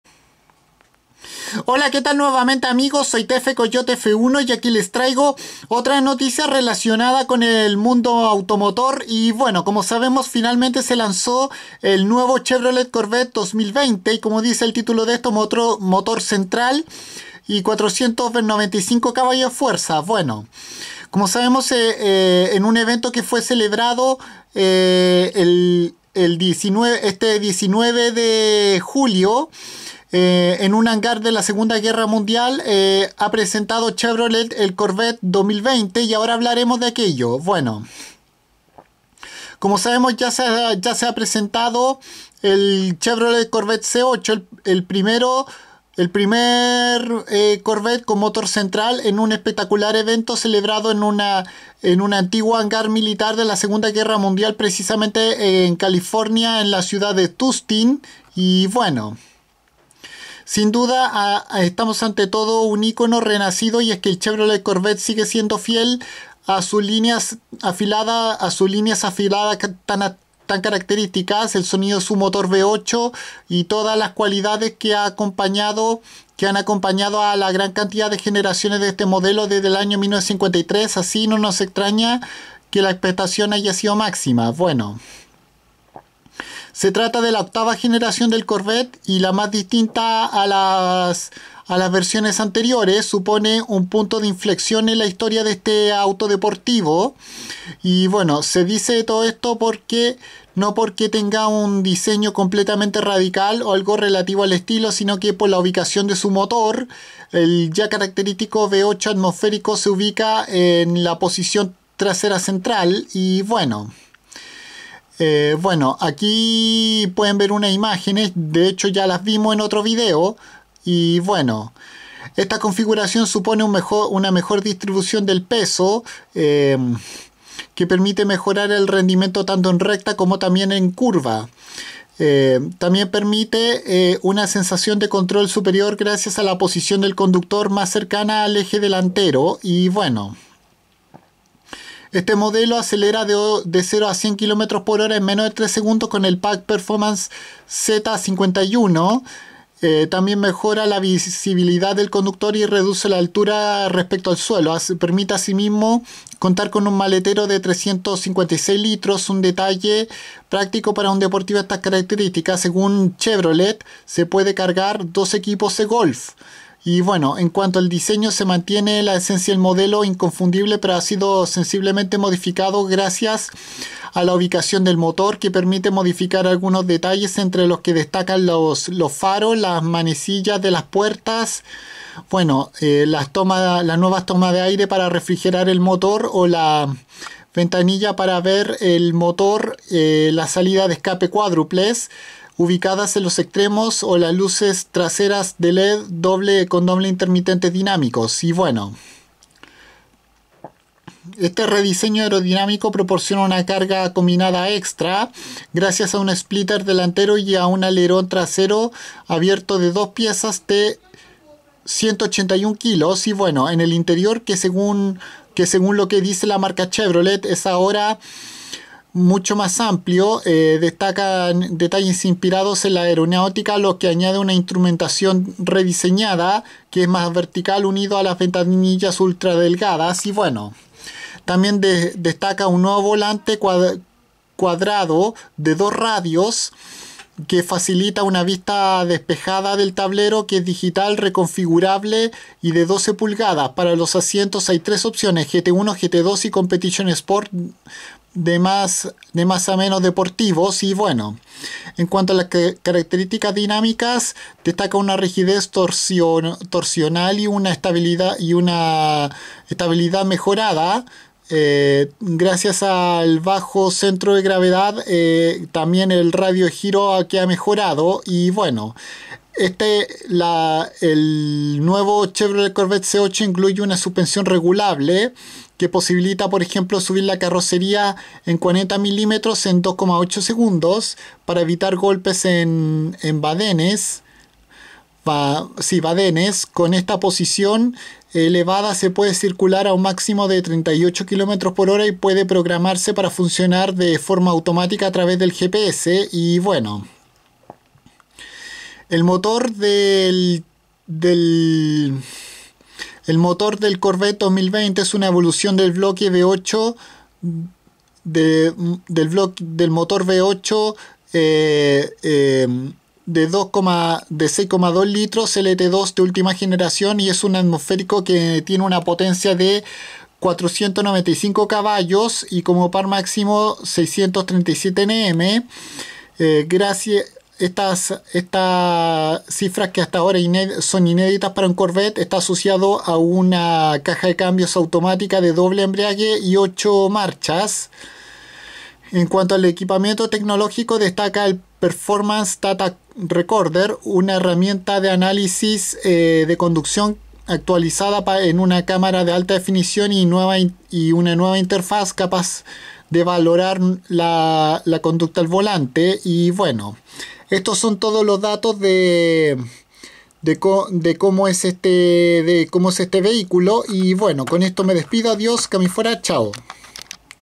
Chevrolet Corvette C8 información
Aquí les cuento como es el primer Corvette con motor trasero central ademas ser el de la octava generación de este legendario superdeportivo norteamericano